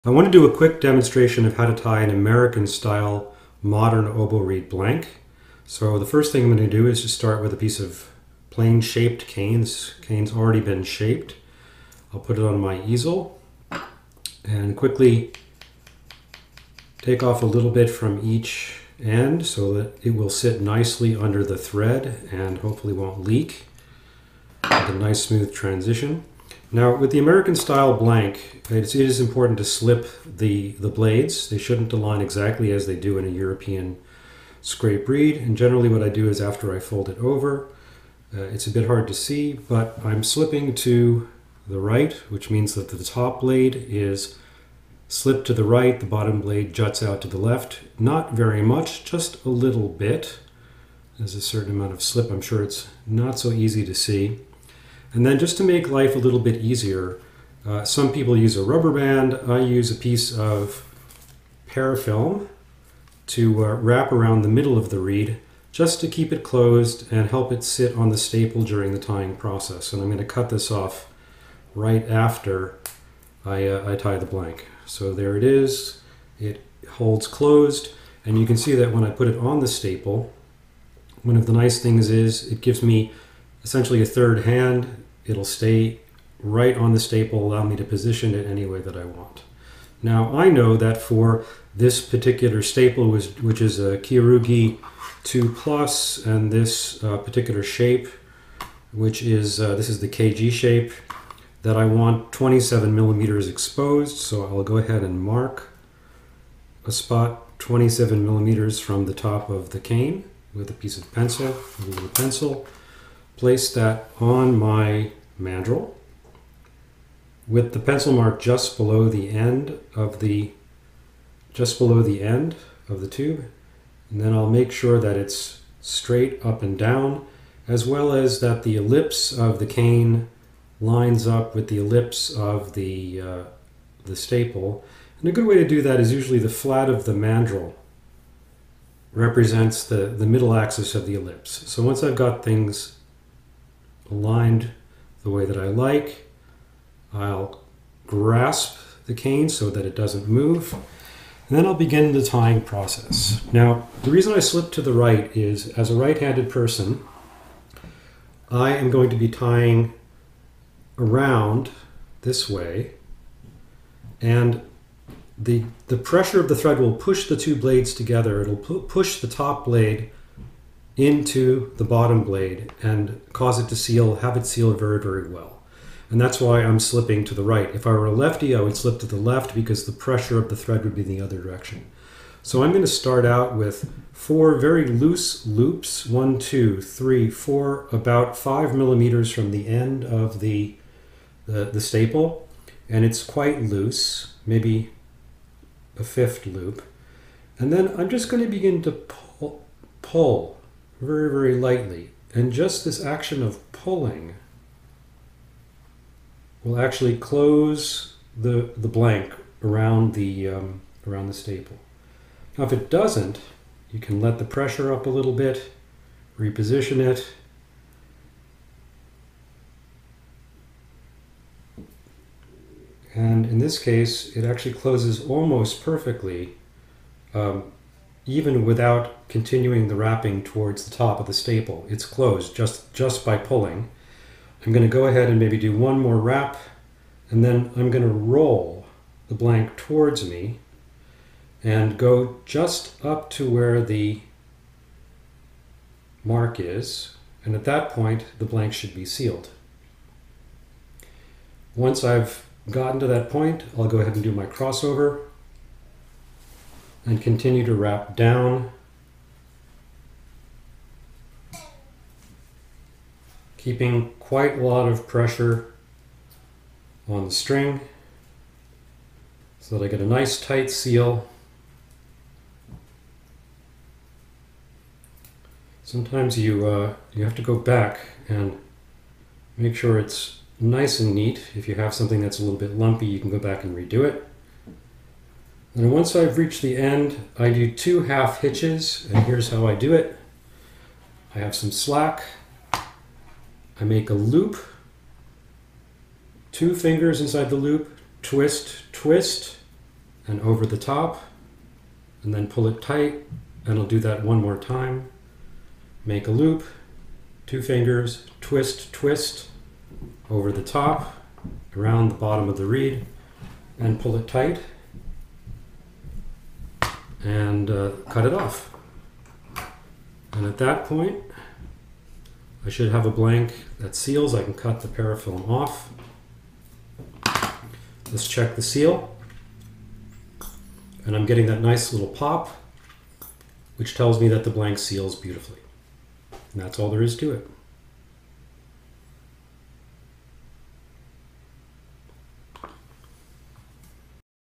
Tying the American Style Oboe Blank
This video is a short demo of tying an oboe reed blank, slipped to the right in the American style